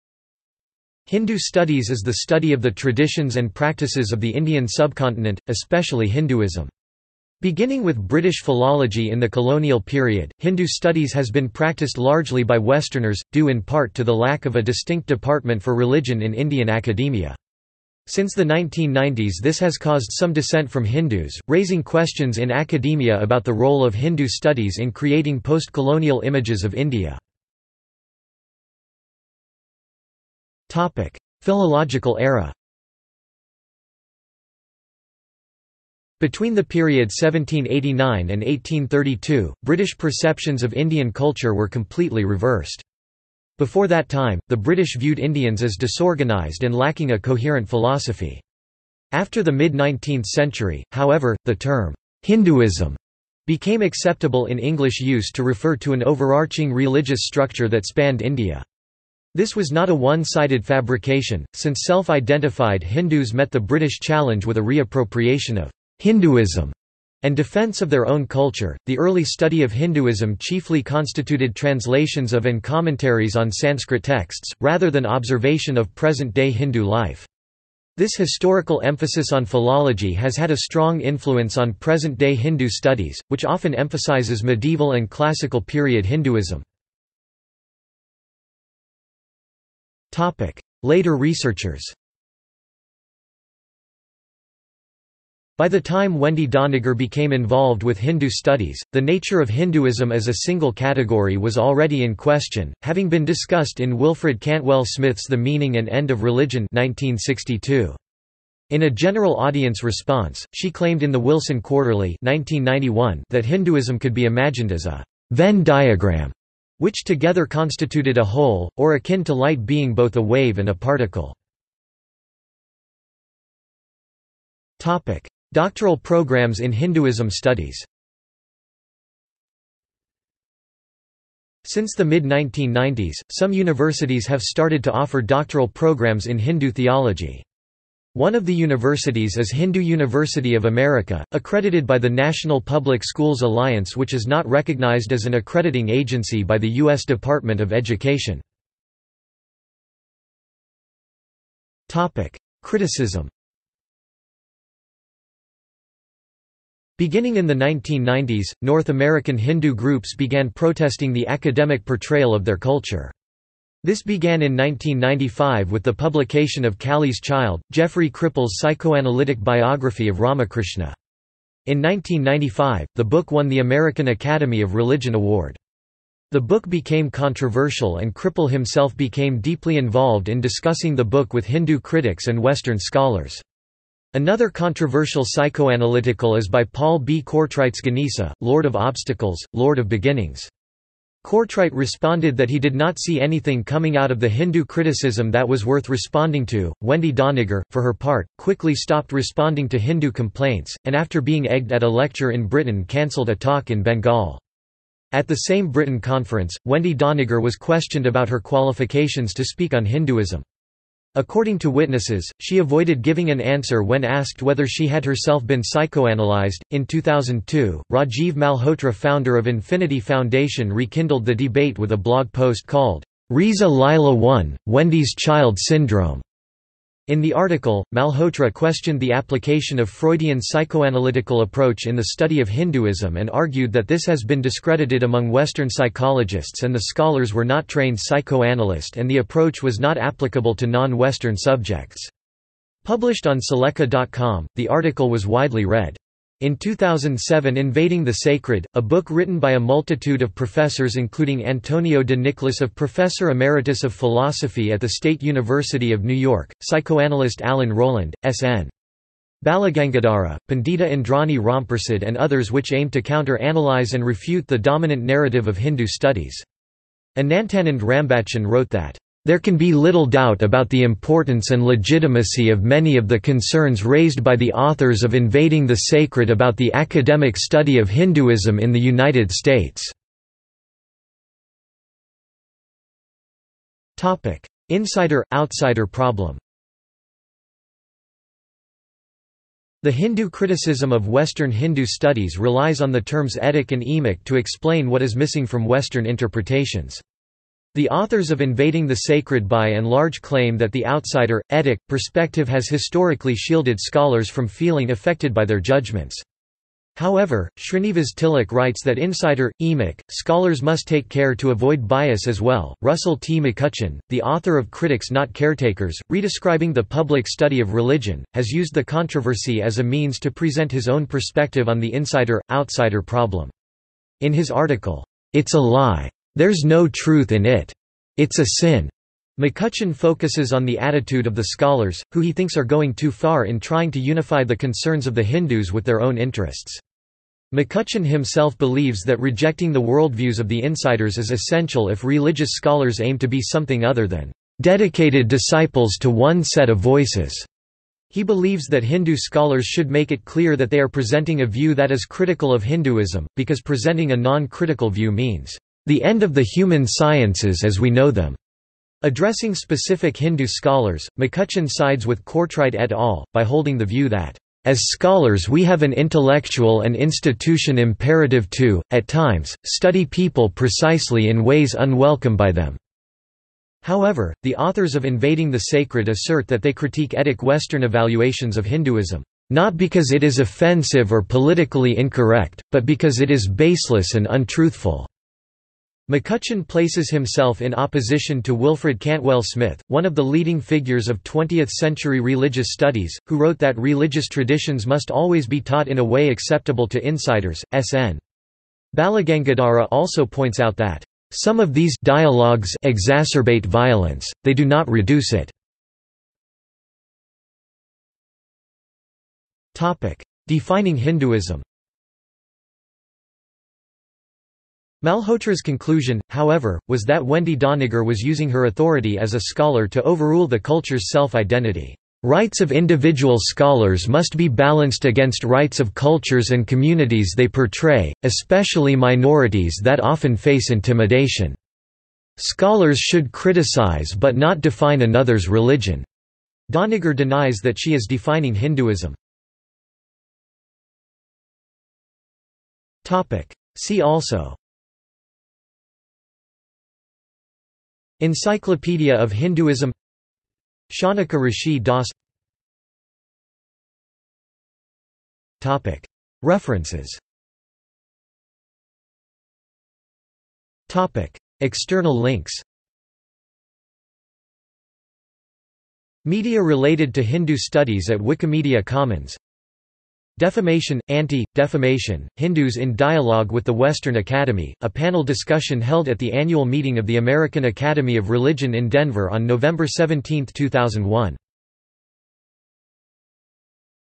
Hindu studies | Wikipedia audio article
This is an audio version of the Wikipedia Article:
Hindu studies

- Socrates

SUMMARY
Hindu studies is the study of the traditions and practices of the Indian subcontinent, especially Hinduism. Beginning with British philology in the colonial period, Hindu studies has been practiced largely by Westerners, due in part to the lack of a distinct department for religion in Indian academia. Since the 1990s this has caused some dissent from Hindus, raising questions in academia about the role of Hindu studies in creating postcolonial images of India.